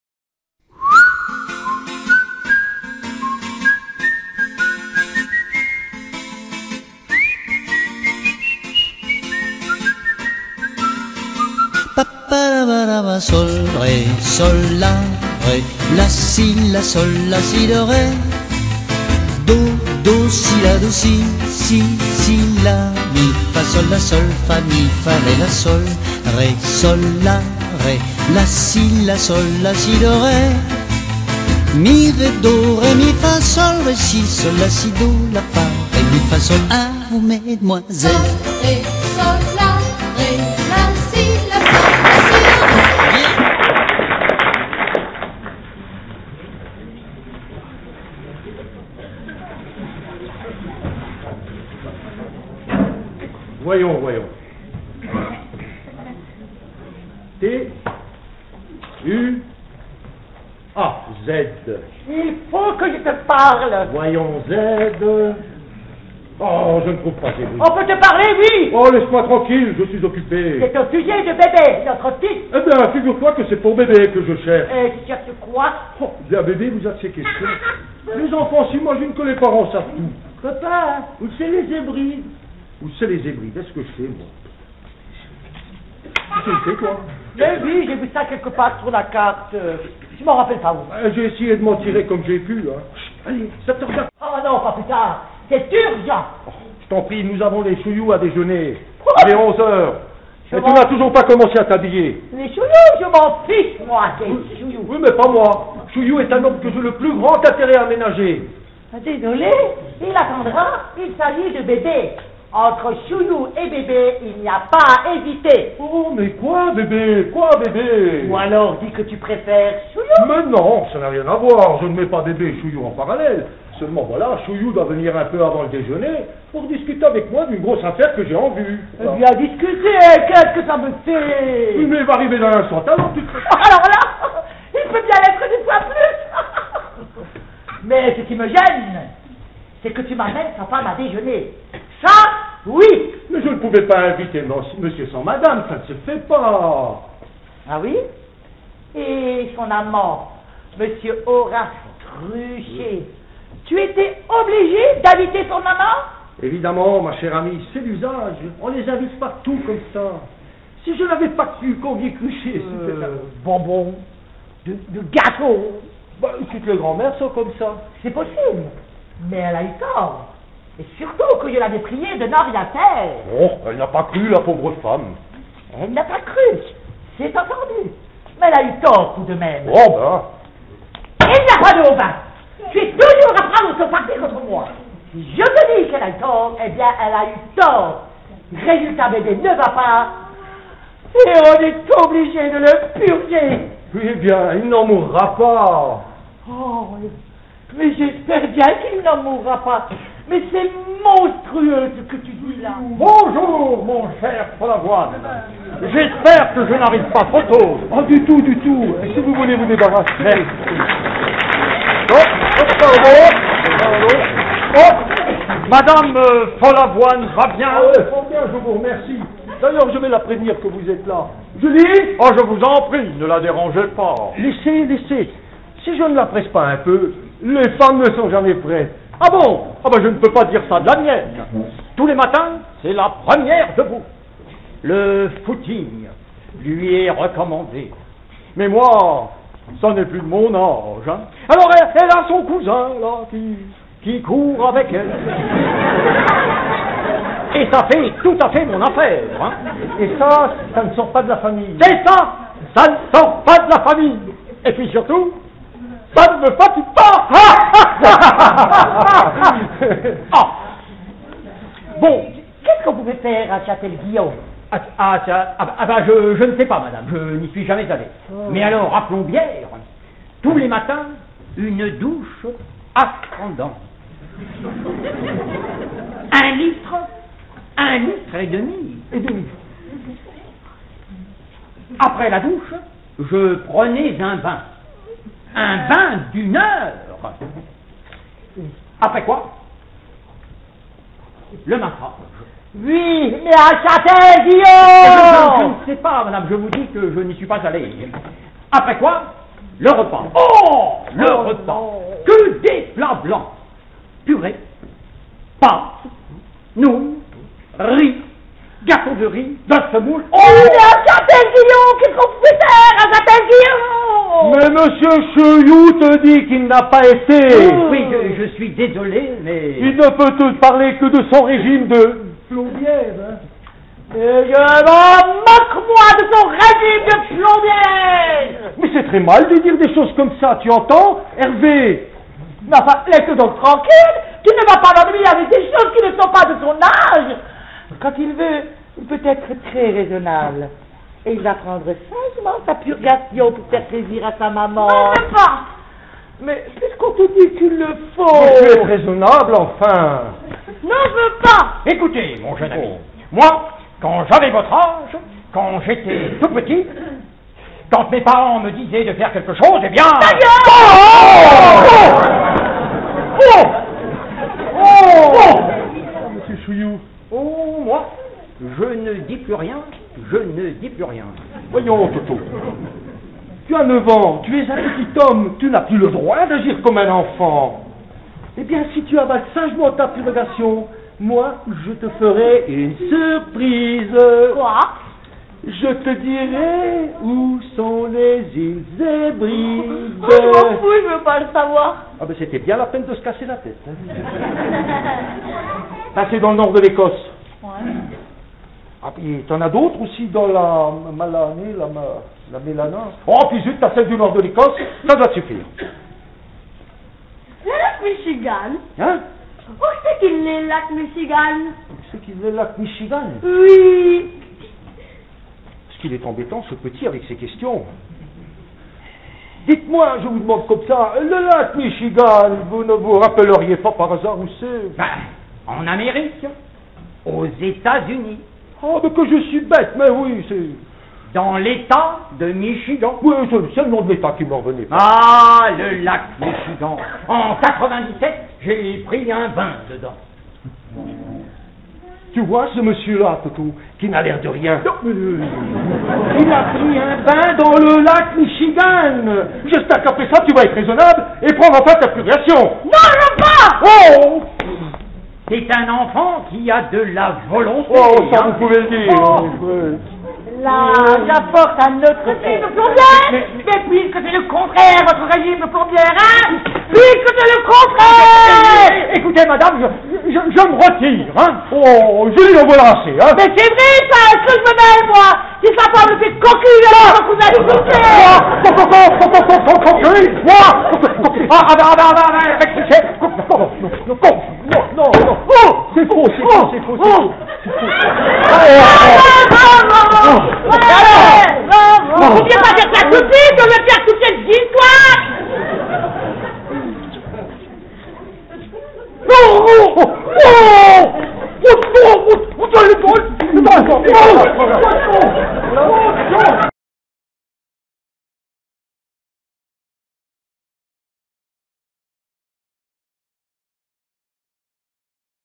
On purge bébé (version courte)
La troupe COTE COUR donne son spectacle le 29 mai 2010 au foyer Emile Huchet à St Avold